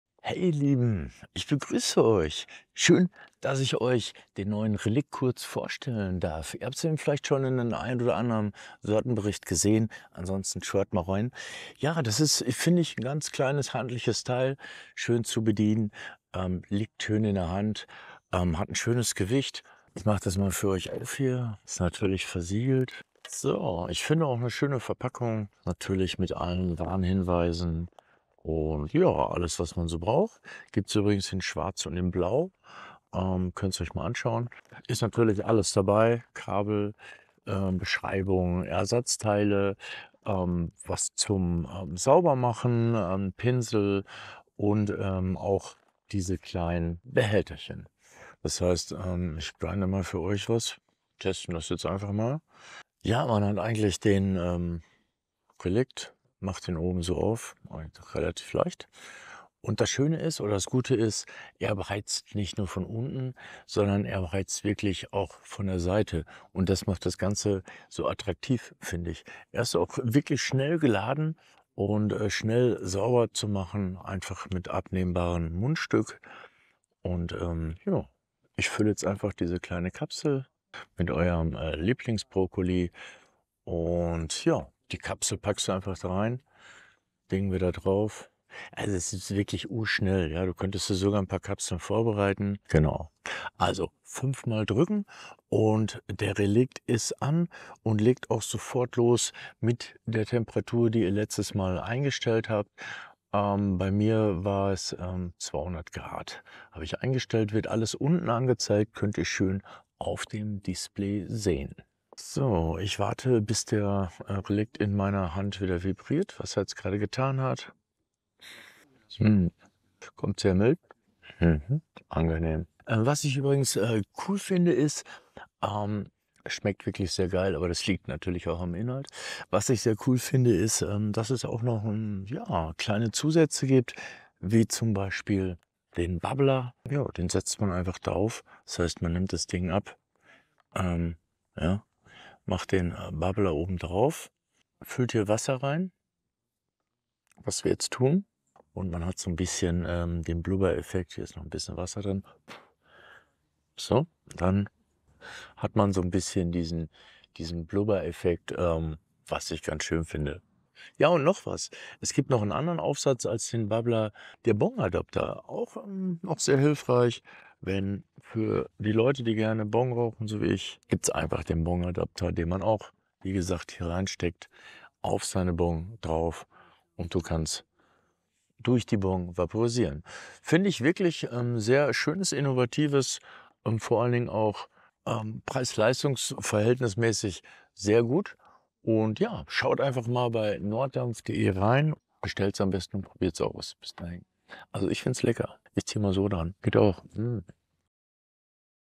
Rauchst du noch oder Verdampfst du schon? Norddampf RELICT Vaporizer 🌿
Willkommen zurück, liebe Vape-Enthusiasten! In diesem Video präsentieren wir den brandneuen RELICT Vaporizer von NORDDAMPF. Tauche ein in die Welt des puren Aromas und des dichten Dampfes, die durch eine innovative Dual-Heat-Technologie ermöglicht wird.

Zum Norddampf Relict ⬇️💚

Hauptmerkmale:

🌡️ Hochpräzise Temperatureinstellung (160-240°C) – Passe die Temperatur deinen Kräutern individuell an und genieße den ultimativen Geschmack.
🎒 Kompaktes Design (30,5 x 30 x 124,1mm) – Perfekt für den Einsatz unterwegs.
🔋 Starke Energiekapazität (2.300mAh) mit Schnellladeverfahren über USB-C. Lade deinen RELICT in knapp 2 Stunden komplett auf.
🌬️ Aroma-Glasmundstück – Erlebe einen authentischen Geschmack und ein unverfälschtes Aroma.
📦 Umfangreiches Zubehör – Von einem Aroma-Glasrohr bis hin zum NORDDAMPF-Aufkleber.
Du interessierst dich für weiteres Zubehör? Schau dir die maßgeschneiderten Optionen von NORDDAMPF an, einschließlich Bubbler, Bong Adapter und Grinder.

Schau dir das Video an und erfahre mehr über den RELICT Vaporizer, seine Eigenschaften und wie du das Beste aus deinem Vape-Erlebnis herausholen kannst.